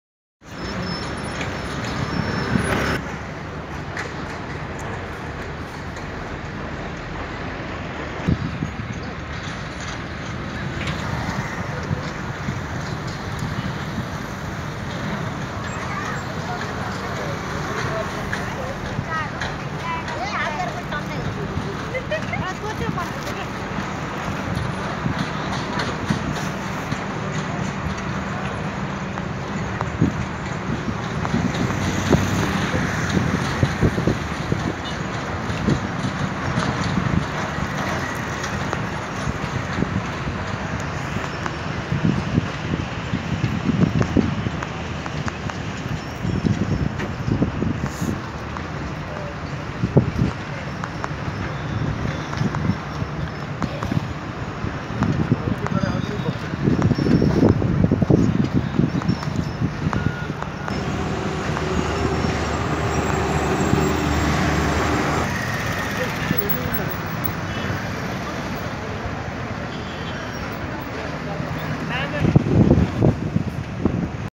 Kalanki UnderPass Bridge Project Live Update | 14-01-2018 Ad- Kathmandu Nepal byChina Aid
Kalanki to thankot Trip | kathmandu nepal | by Sumod